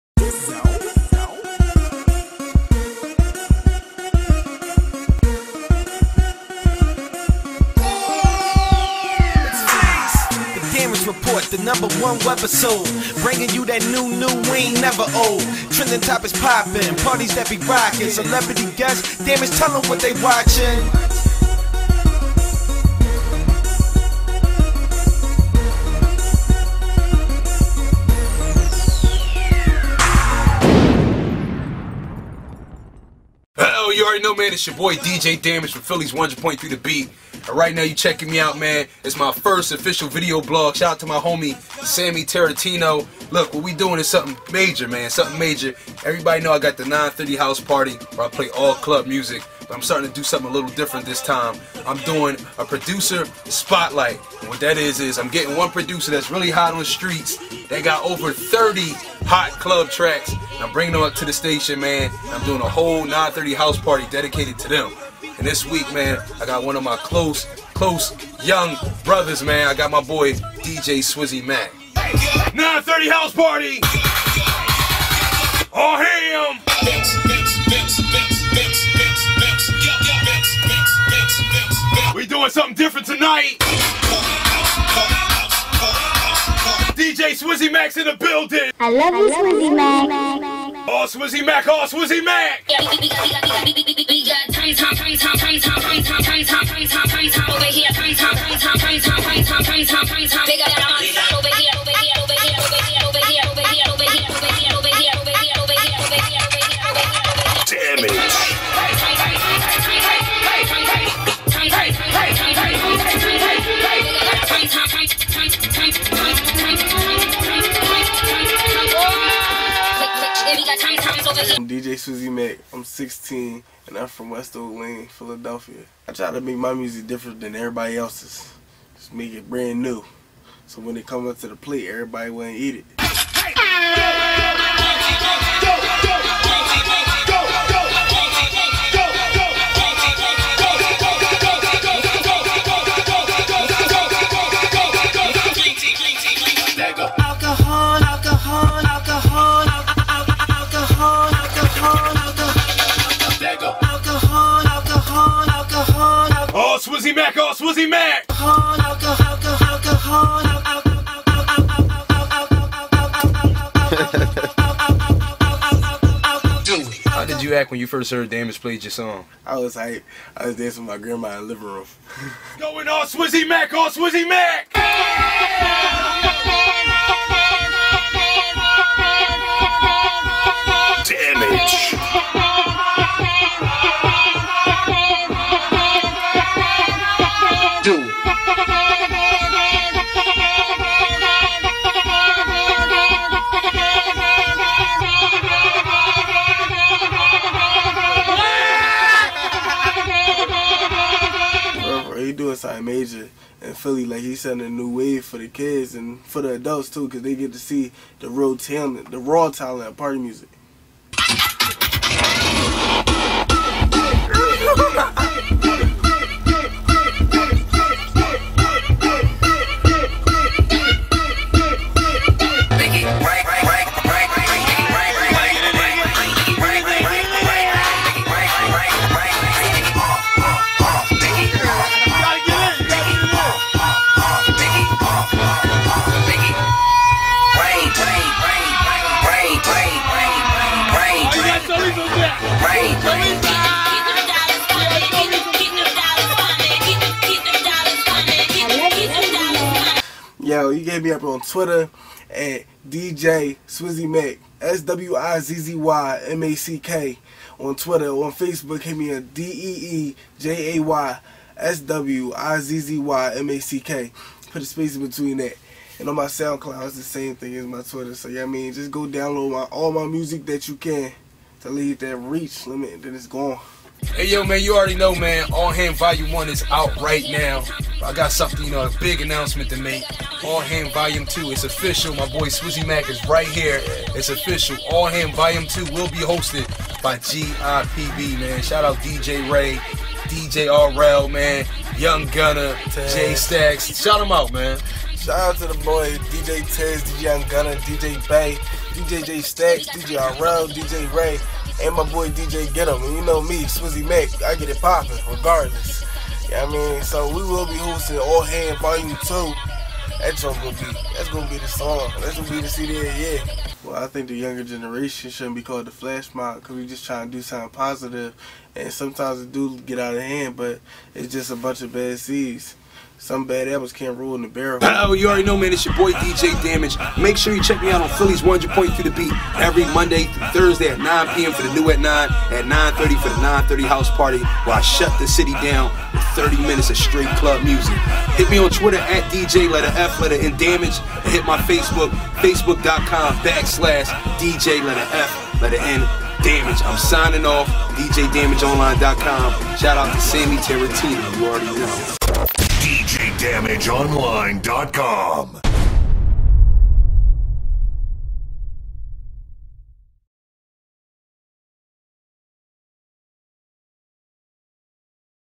DJ DAMAGE VLOG#2 PRODUCER SPOT LIGHT EP.1 THE DAMAGE REPORT
100.3 The Beat's Own DJ DAMAGE
Is putting on for his city, with the club producer spotlight on the 9:30 House Party
Only on 100.3 The beat!

DJ SWIZZY MACK is the first producer to be showcased and here is a 10min podcast preview.

TAKING OVER YOUTUBE AND BRINGING YOU BEHIND THE SCENES OF WHAT IT TAKES TO BE A RADIO DJ AND THE JOURNEY ON HIS WAY TO THE TOP...

THE PRODUCER SPOT LIGHT IS TO SHINE LIGHT ON YOUNG CLUB PRODUCERS AND FIRST UP IS PHILLY'S OWN :DJ SWIZZYMACK